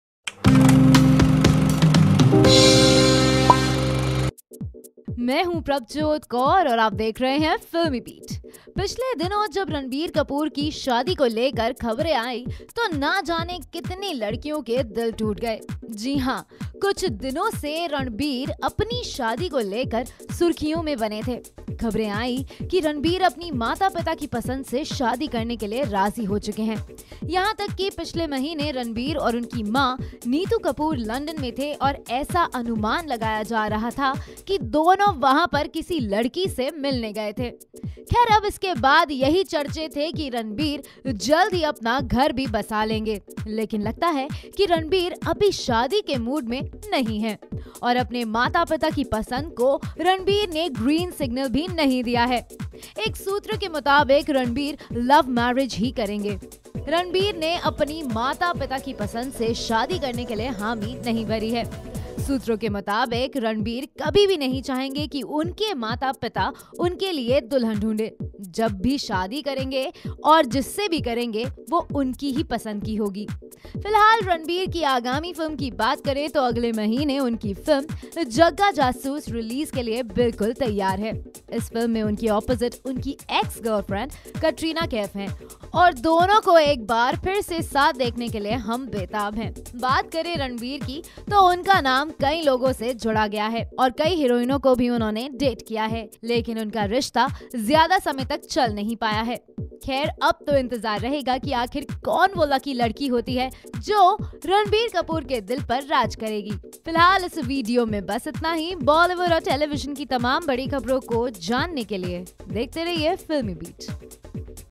Ranbir Kapoor is NOT READY for Arrange Marriage | FilmiBeat
Ranbir Kapoor is not ready for arrange marriage. He believes that he will marry a girl of his choice not his parents Rishi and Neetu Kapoor. Lately, Ranbir was in news that he has gone to London with his mom Neetu Kapoor to meet a girl for marriage. Watch video to know more.